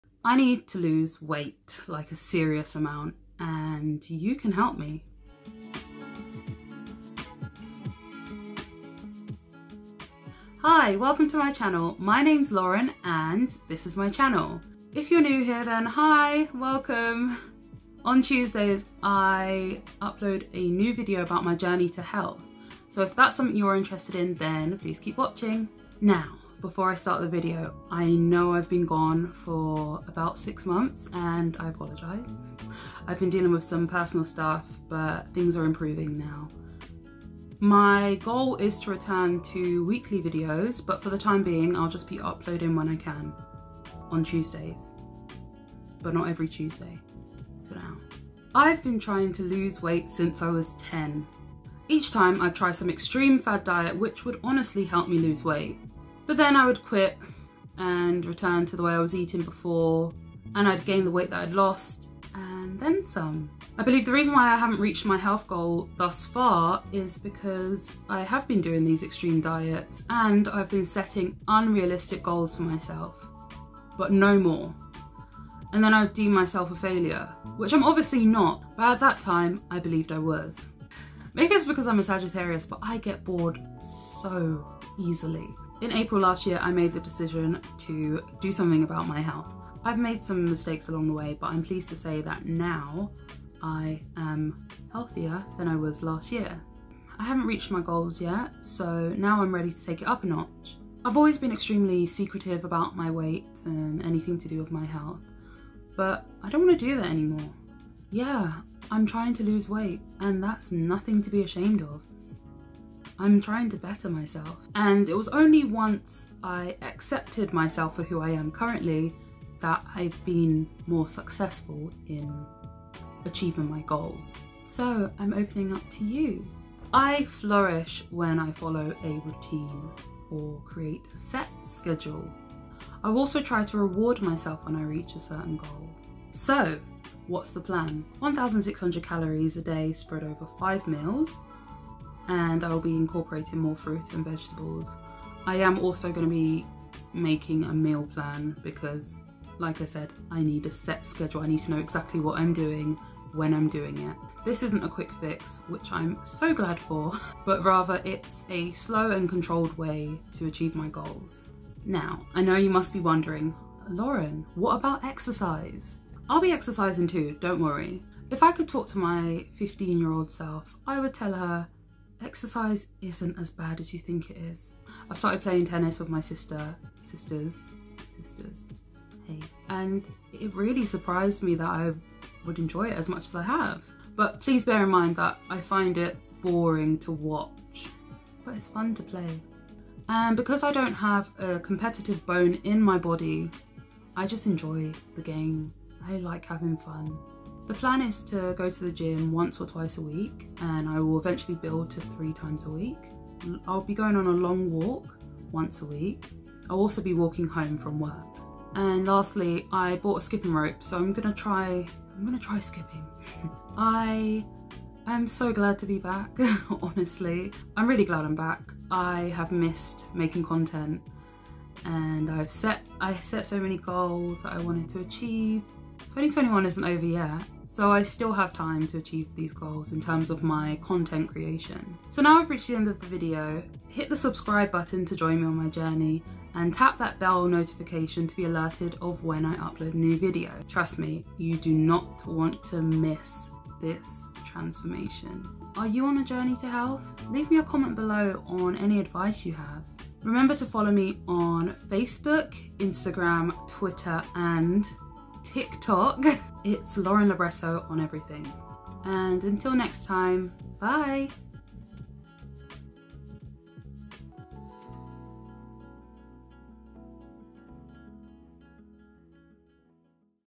I need to lose weight | Lauren Libretto
Hi there,
I'm finally back after a long hiatus. In this video I am talking about how I am planning to lose weight and take control of my life.

Come and join me on my other social platforms